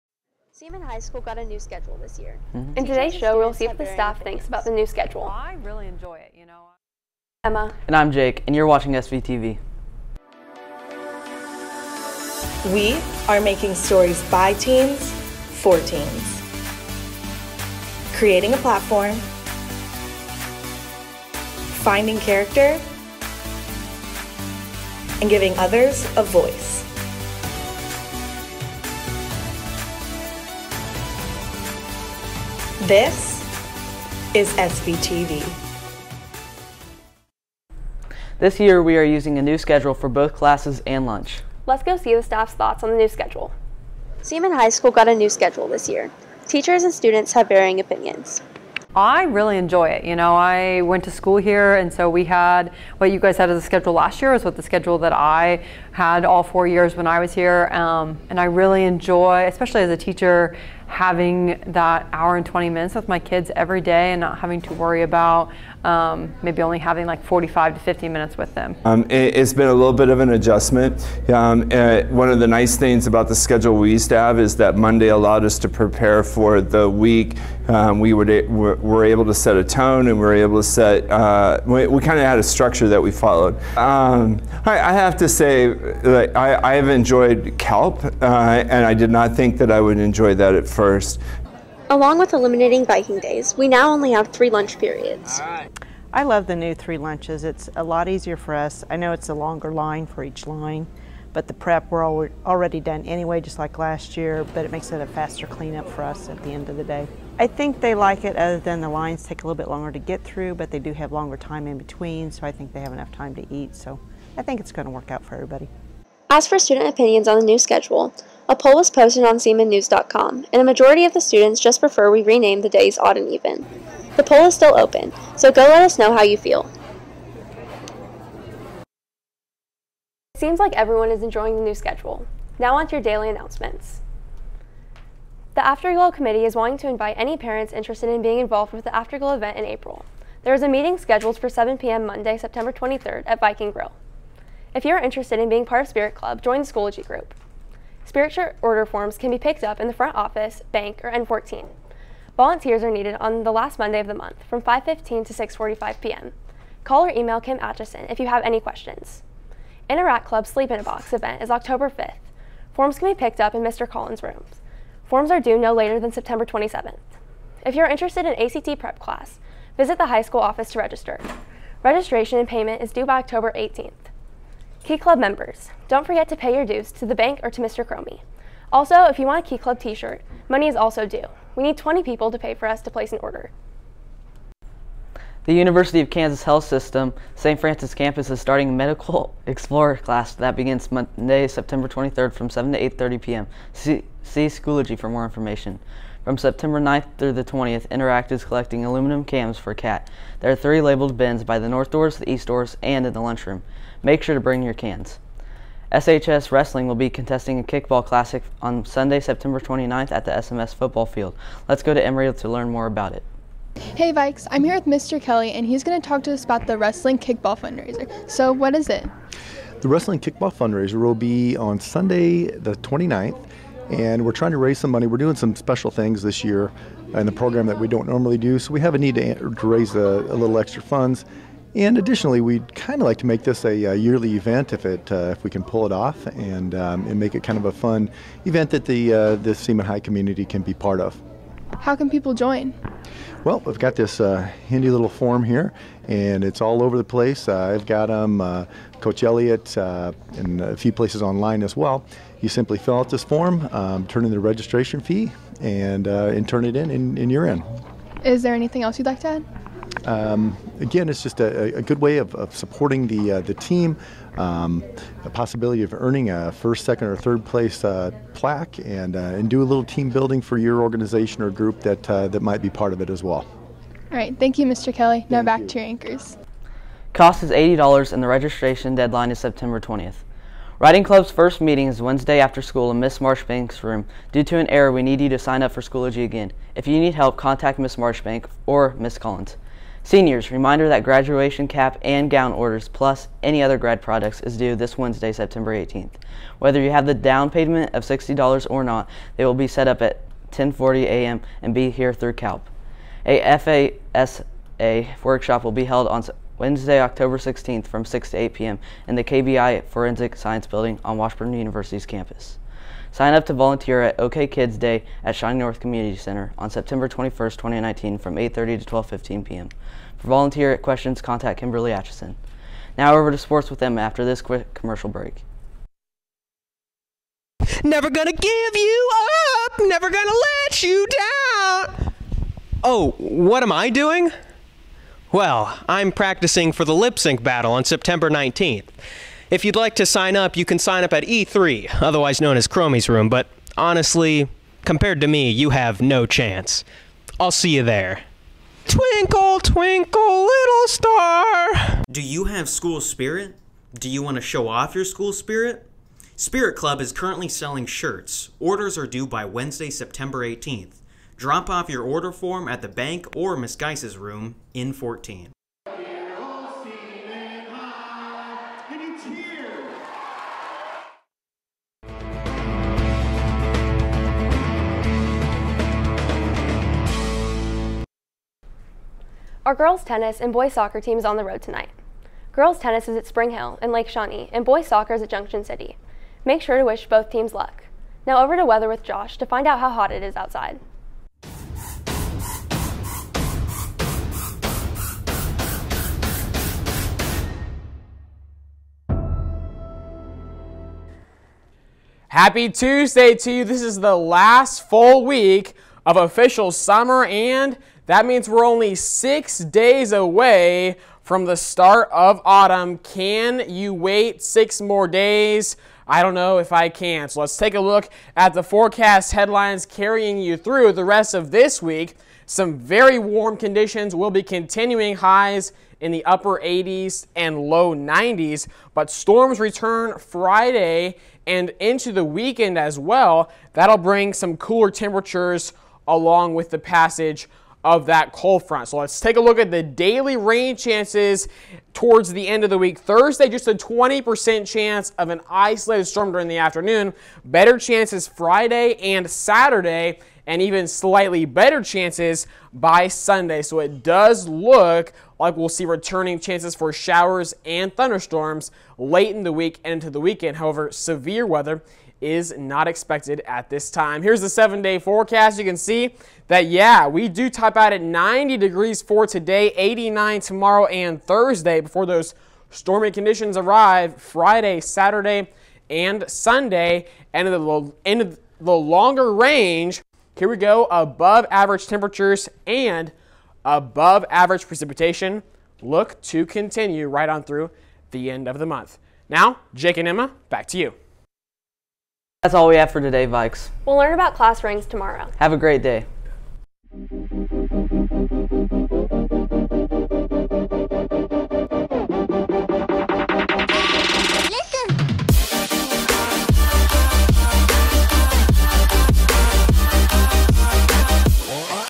9-17-19 SVTV Daily News
SVTV Daily News
Stories: Kickball and New Schedule
Anchors: Jake & Emma
Weather: Josh
Director: Jake